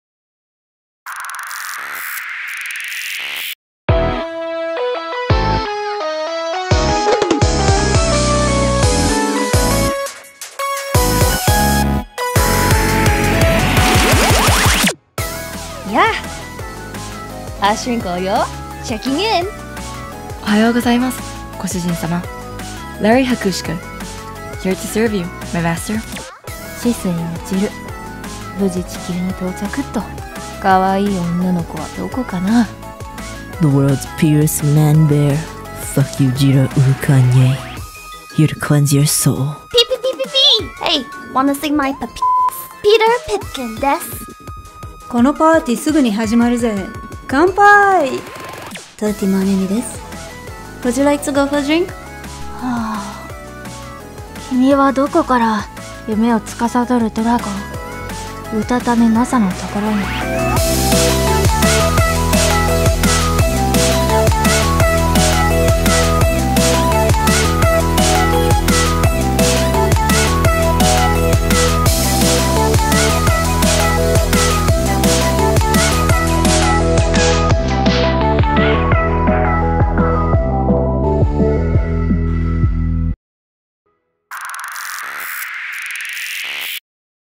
Phase Connect Gen.2 Debut Trailer [April Fools Edition]
How are these fine gentlemen for Gen.2?

Re-upload of our April Fools debut trailer featuring

@RinkouAshelia  as Ash Rinkou
@PipkinPippa  as Peter Pipkin
@ShisuiMichiru  as Michiru-Kun
@UtataneNasa  as Nasa-Kun
@TenmaMaemi as 30ma Manemi
@FujikuraUruka as Urukanye Fukujiraye
@HakushikaIori as Larry Hakushika